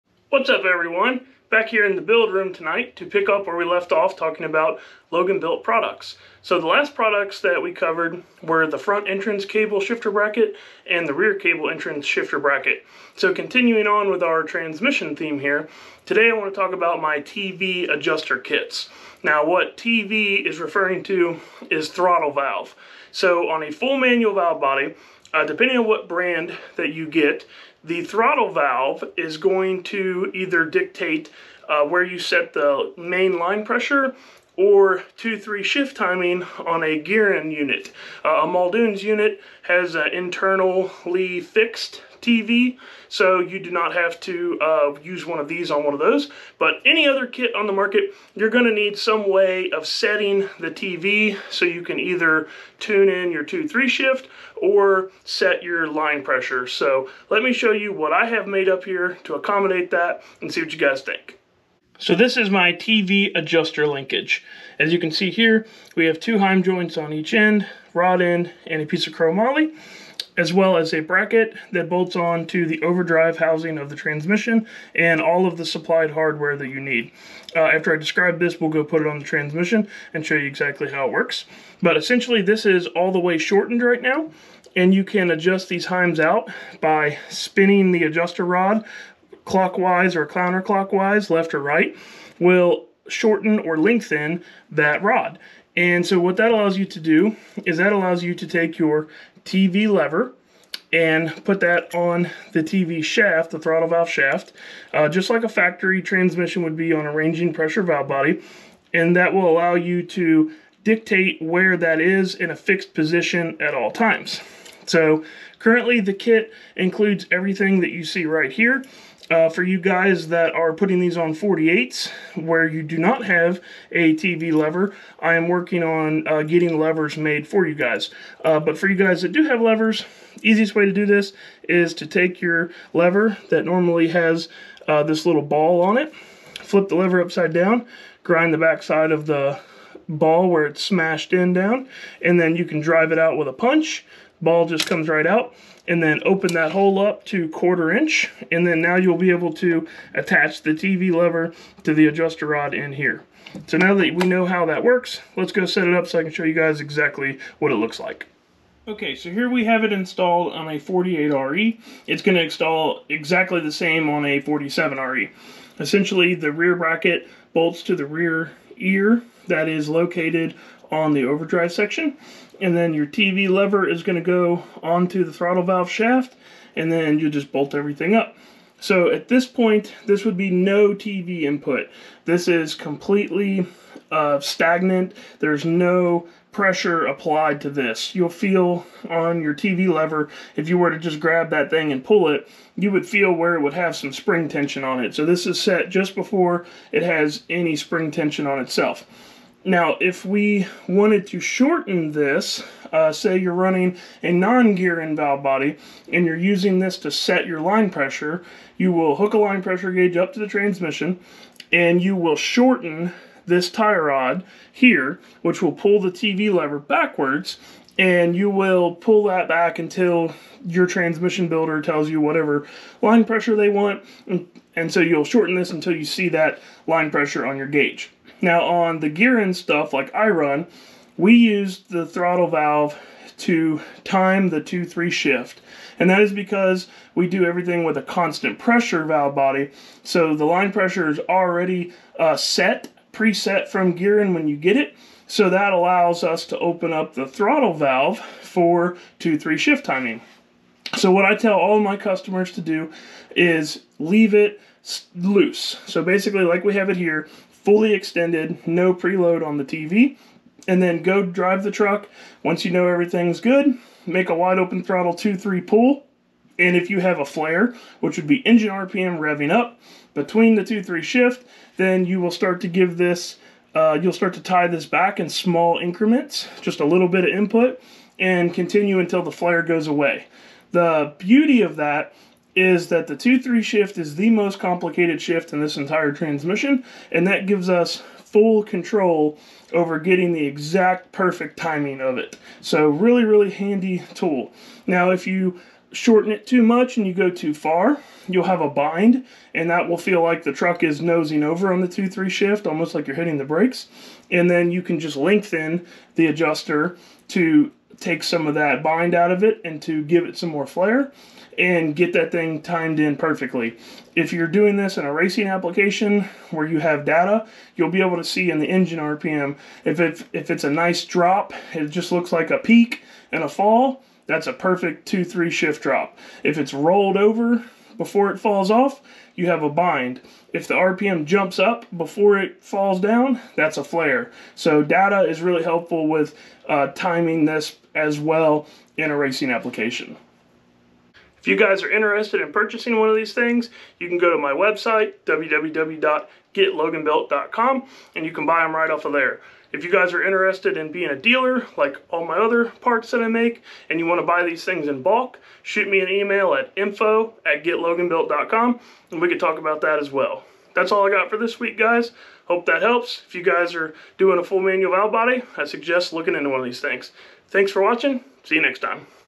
Loganbuilt Full Manual Valve Body TV Adjuster Kit! Why You NEED This!
Product highlight of my FMVB TV adjuster kit!